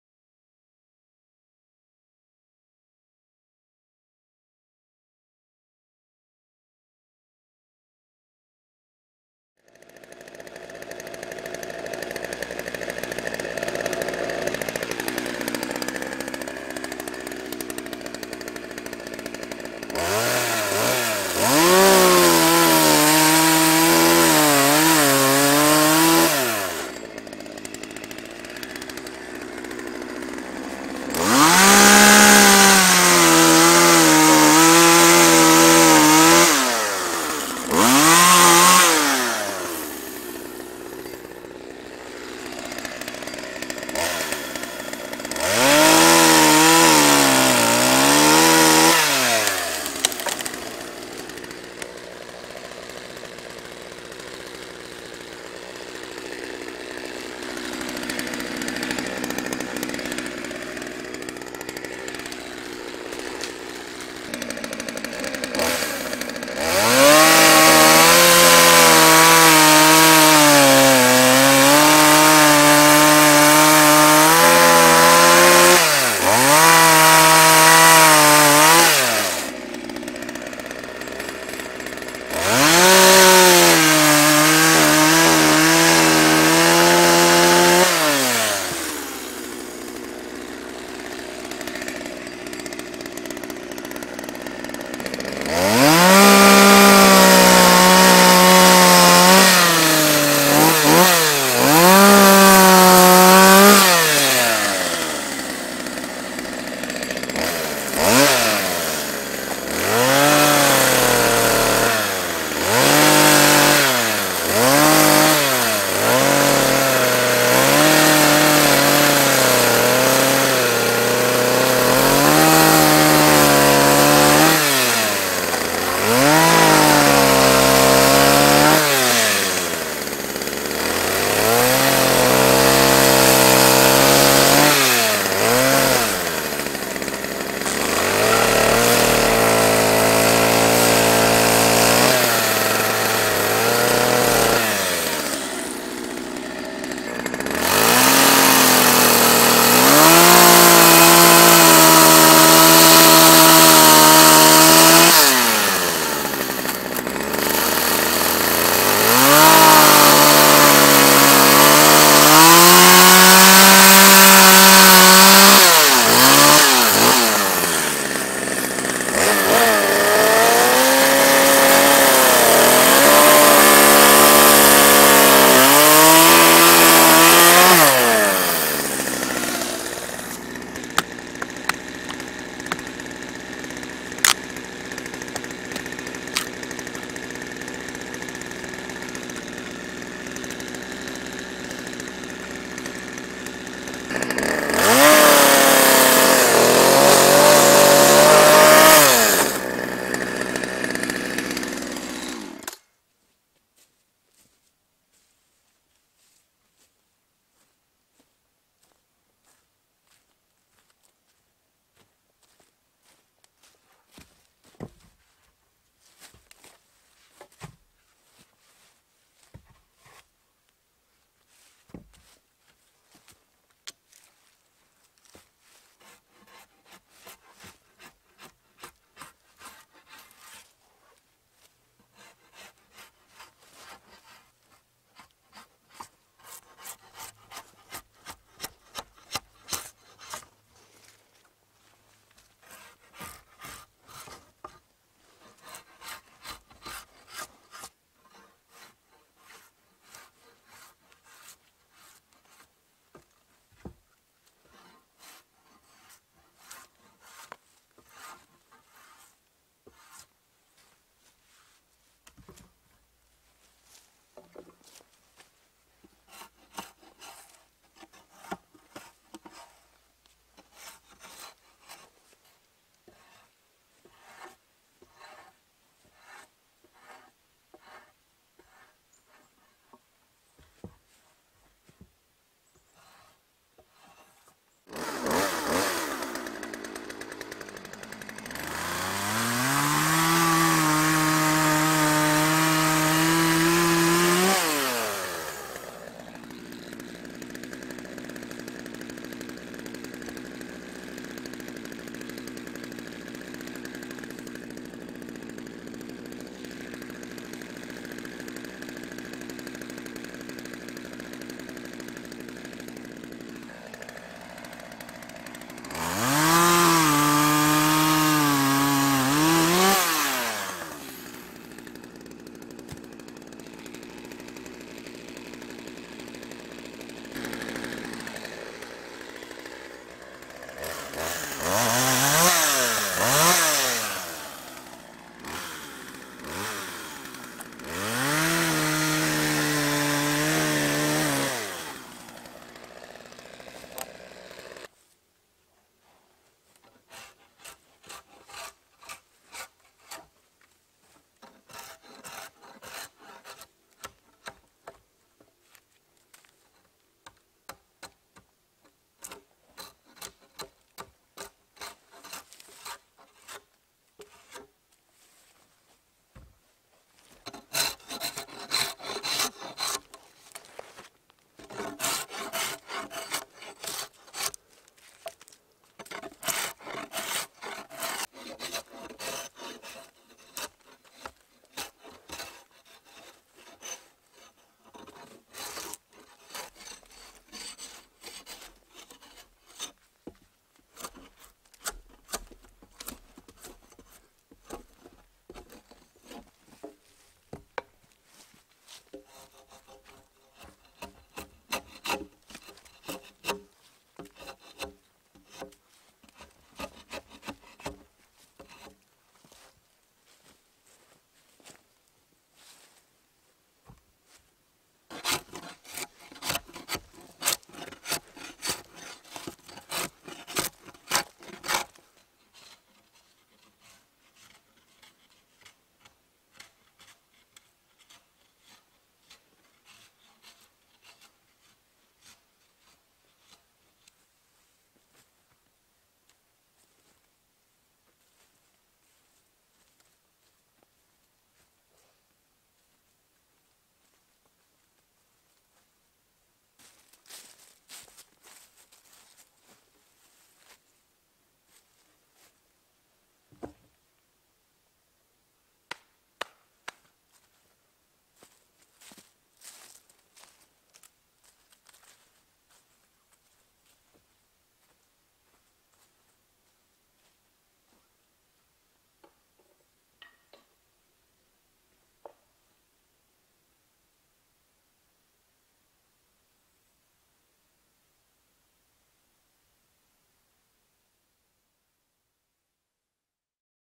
Bushcraft Outpost - Natural Debarking station
As part of this new series, the hope is to make more aesthetically pleasing structures as well as functional. The debarking station will work towards this. In this episode I design a natural station for future projects and test it out. This design is inspired by DonVon Gun, who has a wonderful channel.

Gear:

Mic - Rode Micro
ND Filter - Kennel
Tripod - Optex Trident